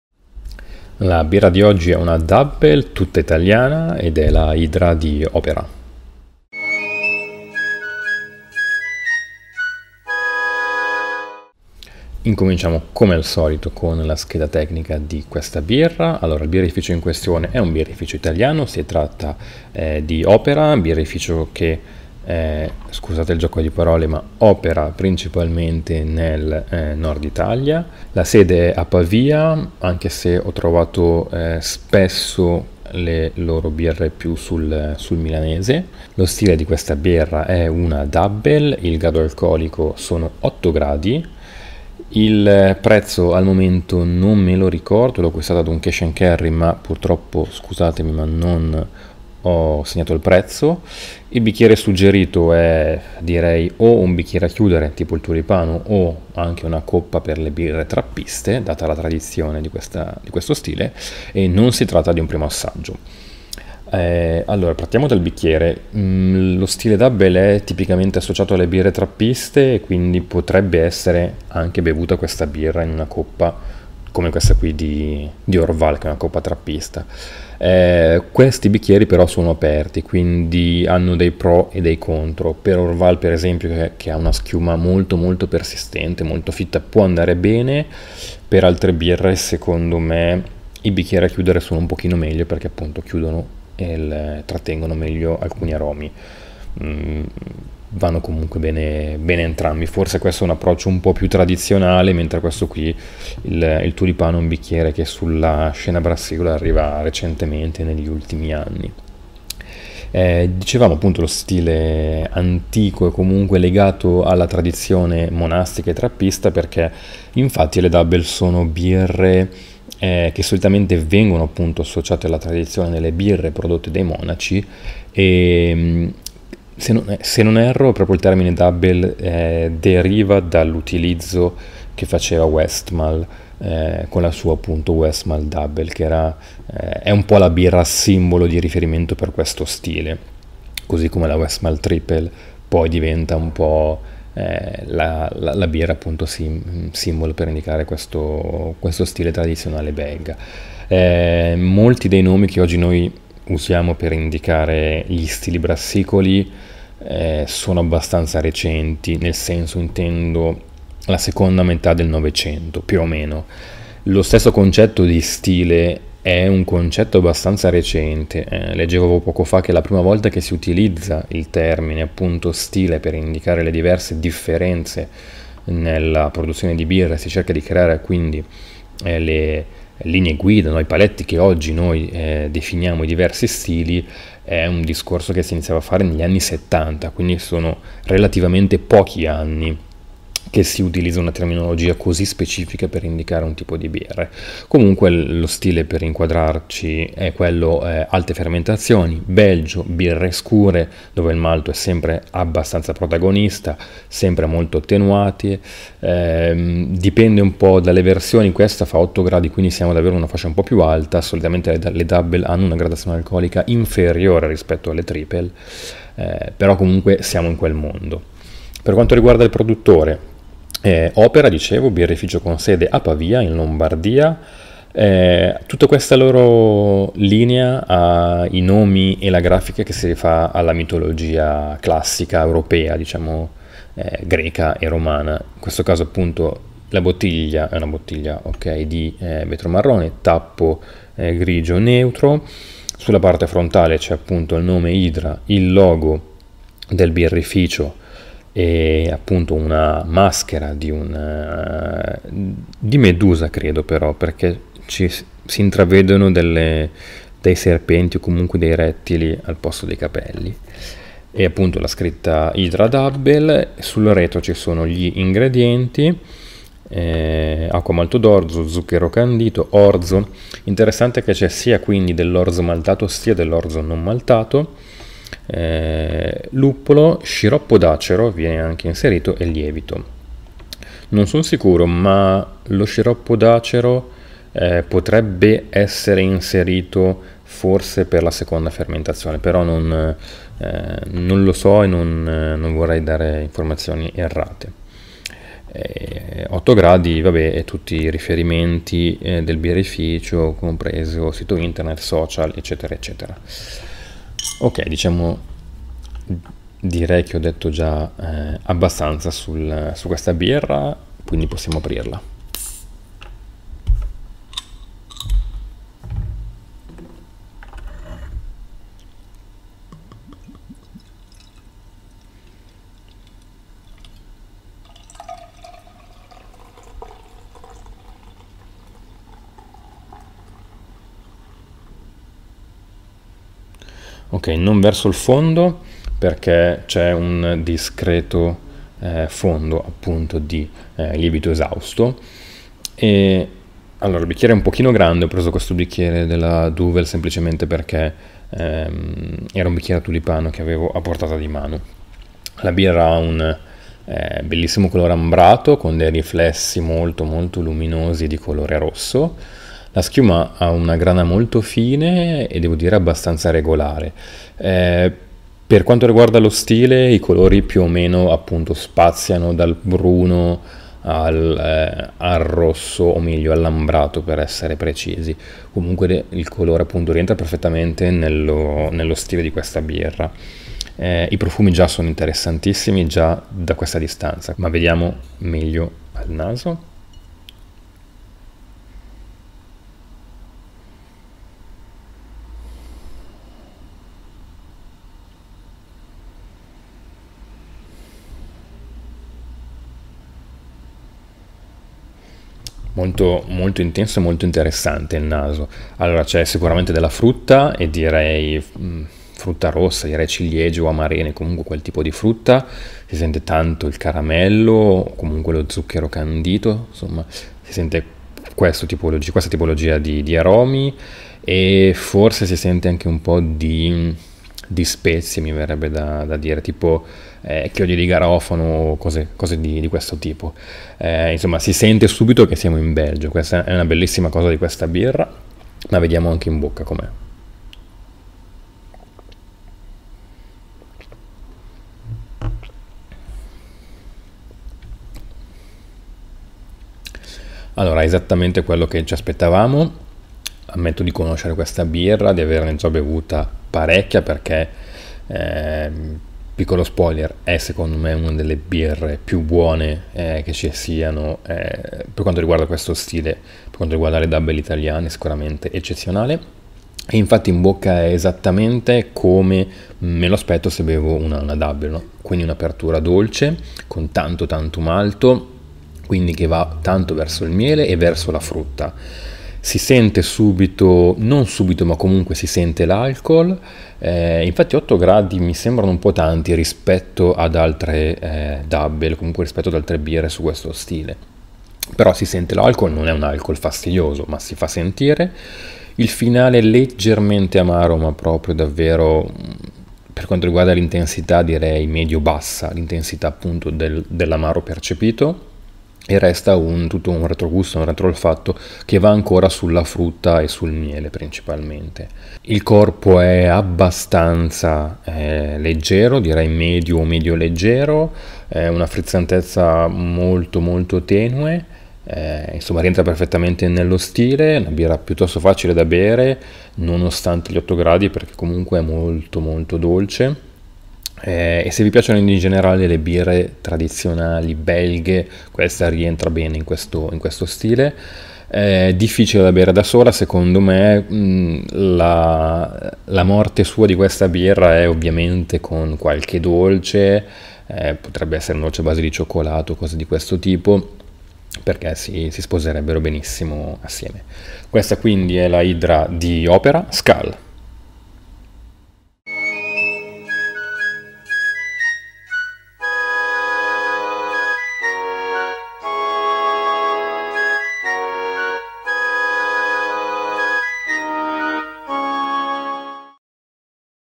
SKAL! Opera HYDRA, una dubbel italiana - Degustazione #28
Il birrificio Opera (Pavia) ha nella sua linea principale questa ottima dubbel, uno stile tradizionalmente belga.

"Skal" (skål, skál, o a volte anche nella sua variante "skoal") è una parola in norreno antico, la lingua parlata dai popoli del nord Europa che oggi chiamiamo, genericamente, vichinghi. Veniva pronunciata ogni qualvolta si faceva un brindisi, alzando la coppa di legno o il corno potorio.